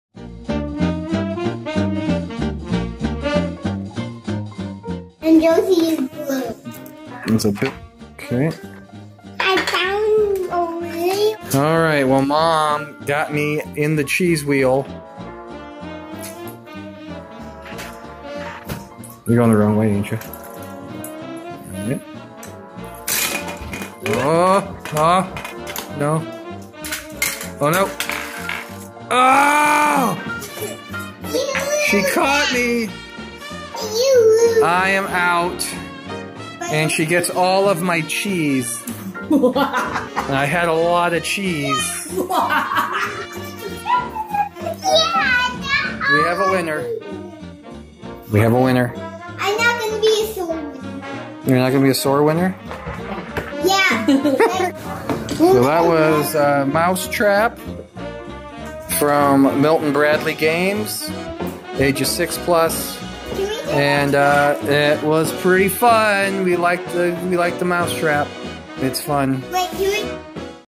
Board games with kids: Mouse Trap!
I play board games with my kids and in this episode we play the classic game Mouse Trap from Milton Bradely for ages 6+.